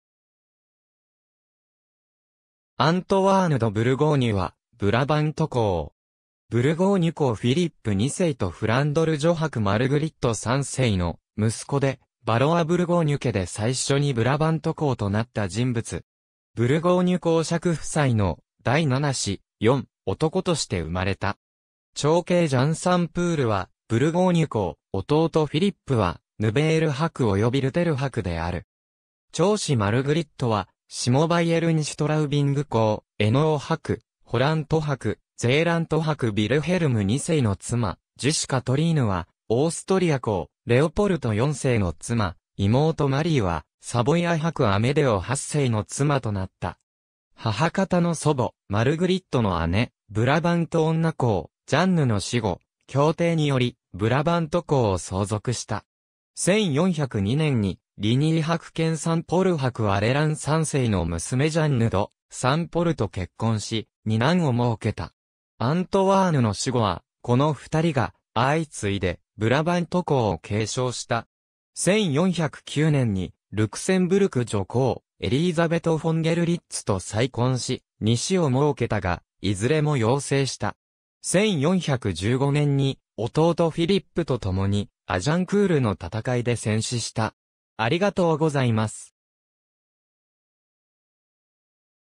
アントワーヌ (ブラバント公)
ヴァロワ＝ブルゴーニュ家
アントワーヌ・ド・ブルゴーニュ（Antoine de Bourgogne, 1384年8月 - 1415年10月25日）は、ブラバント公（在位：1406年 - 1415年）。
ブルゴーニュ公フィリップ2世（豪胆公）とフランドル女伯マルグリット3世の息子で、ヴァロワ＝ブルゴーニュ家で最初にブラバント公となった人物。
ブルゴーニュ公爵夫妻の第7子、四男として生まれた。
長兄ジャン・サン・プールはブルゴーニュ公、弟フィリップはヌヴェール伯およびルテル伯である。
長姉マルグリットは下バイエルン＝シュトラウビング公・エノー伯・ホラント伯・ゼーラント伯ヴィルヘルム2世の妻、次姉カトリーヌはオーストリア公レオポルト4世の妻、妹マリーはサヴォイア伯アメデオ8世（対立教皇フェリクス5世）の妻となった。
母方の祖母マルグリットの姉、ブラバント女公ジャンヌの死後、協定によりブラバント公を相続した。
1402年にリニー伯兼サン＝ポル伯ワレラン3世の娘ジャンヌ・ド・サン＝ポル（? - 1406年）と結婚し、2男をもうけた。
アントワーヌの死後はこの2人が相次いでブラバント公を継承した。
1409年にルクセンブルク女公エリーザベト・フォン・ゲルリッツと再婚し、2子をもうけたが、いずれも夭逝した。
1415年に弟フィリップと共にアジャンクールの戦いで戦死した。